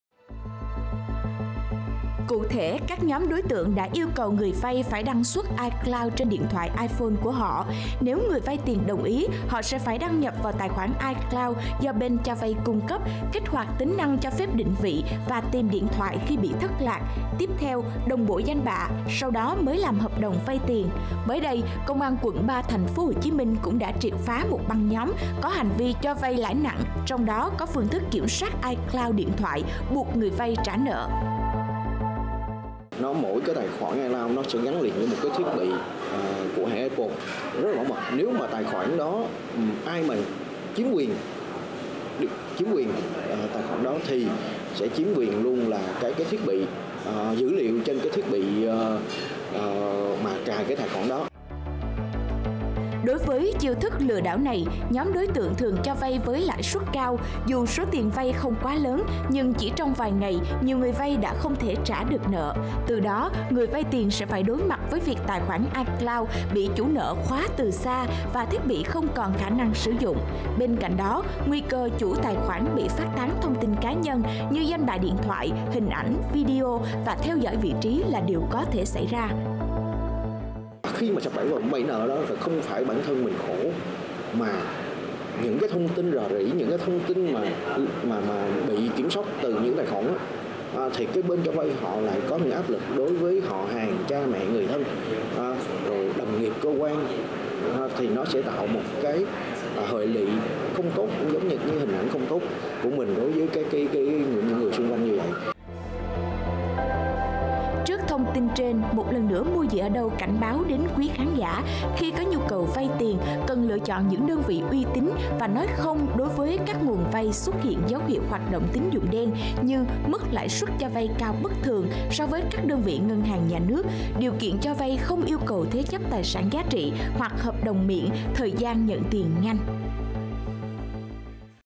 Bẫy vay nặng lãi từ việc chỉ cần thế chấp tài khoản Icloud là vay được tiền siêu nhanh gọn lẹ
Mới đây, Công an quận 3, TPHCM cũng đã triệt phá một băng nhóm có hành vi cho vay lãi nặng, trong đó có phương thức kiểm soát iCloud điện thoại, buộc người vay trả nợ.